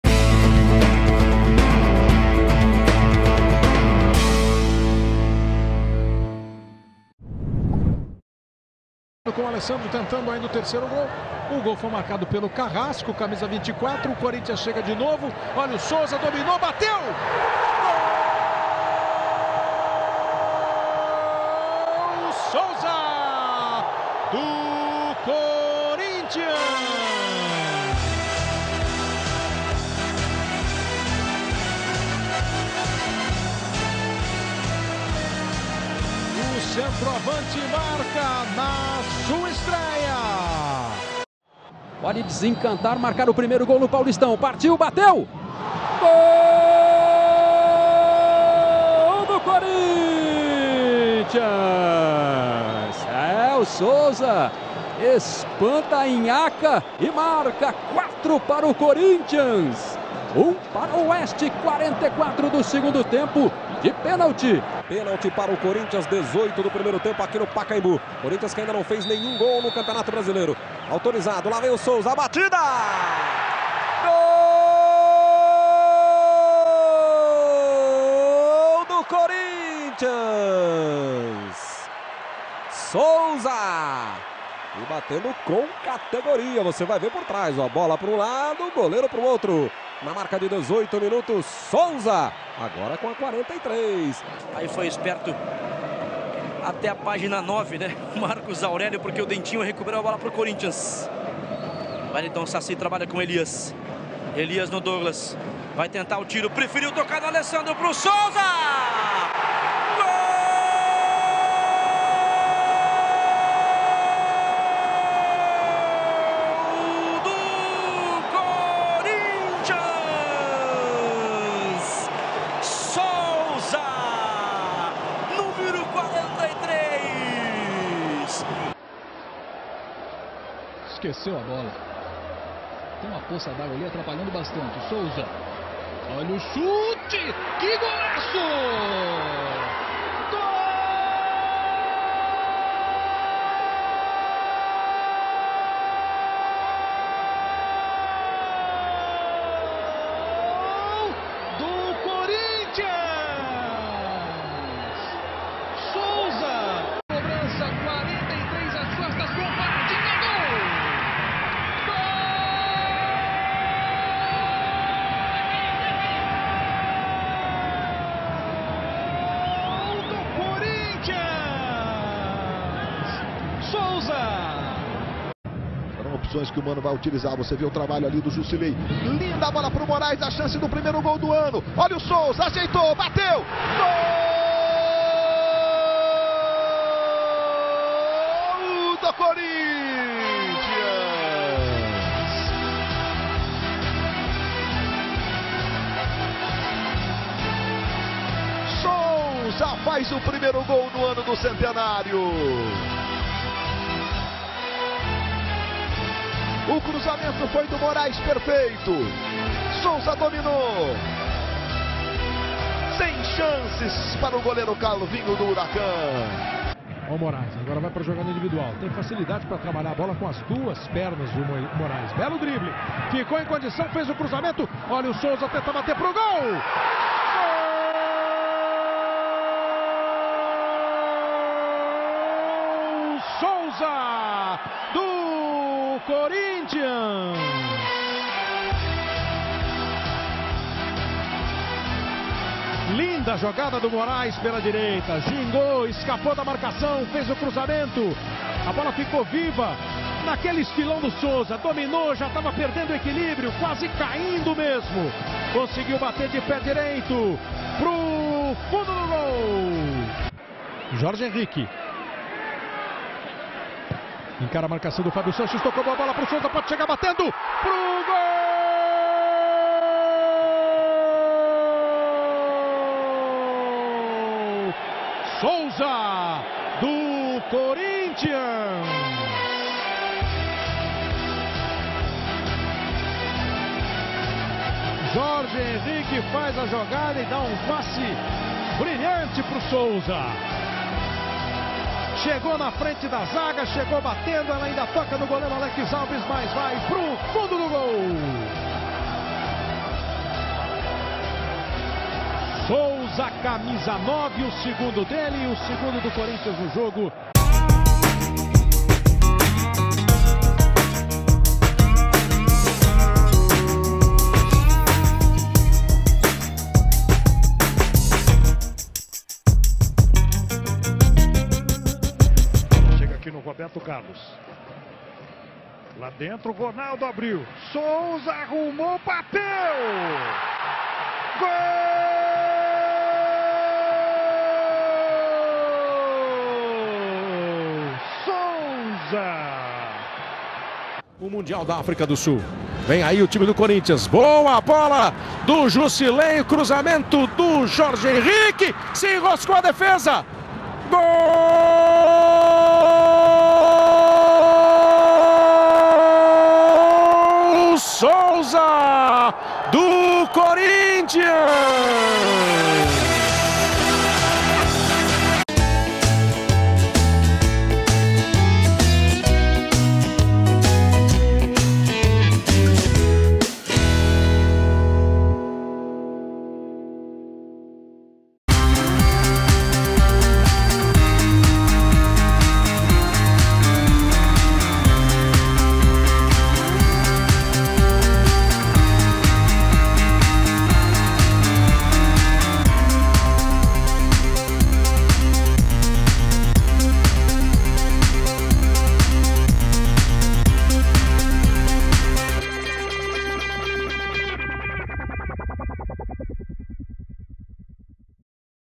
Atacante Souza! TODOS os gols pelo Corinthians!
Todos os gols do atacante Souza com a camisa do Corinthians, entre 2009 e 2010 o jogador jogou 69 jogos e marcou 13 gols.